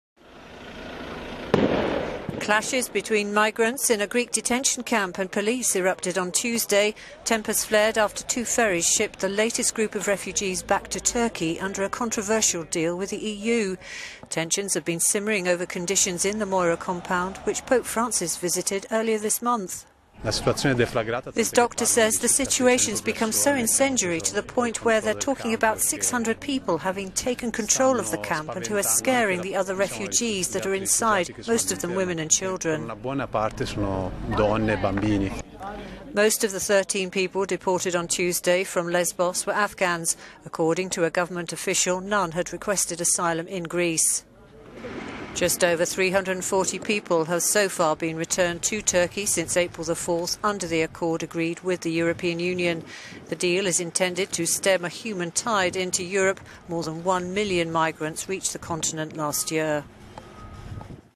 Clashes erupt in Greek migrant camp
Clashes between migrants in a Greek detention camp and police erupted on Tuesday. Tempers flared after two ferries shipped the latest group of refugees back to Turkey under a controversial deal with the EU.

Witnesses said bins were set fire to and rocks and stones were thrown at police. About 200 youths broke through a partition in the camp. Riot breaks out at refugee camp on Greek island of Lesbos